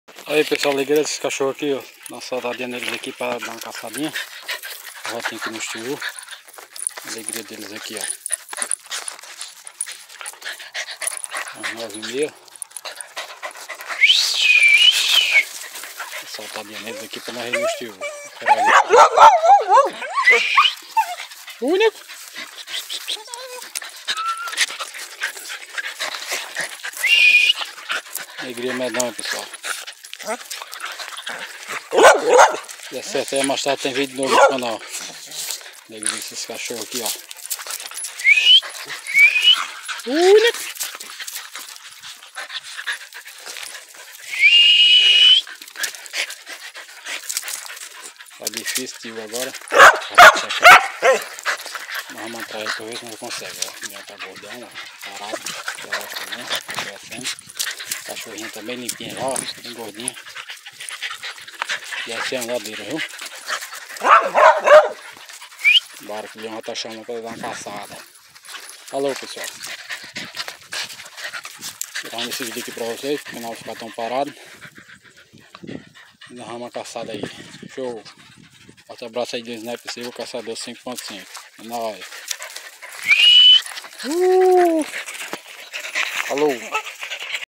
saindo pra caçada de lagarto teiu com os cachorros.
saindo para caçada de lagarto Teiú com cachorro leão e Feroz e a cachorrinha desceu vídeo mostra alegria dos meus cachorros de caça ao sair para caçada de lagarto teiú Agradeço a todos vocês por assistir esse vídeo e um forte abraço do Sniper Silva Caçador 5.5 valeu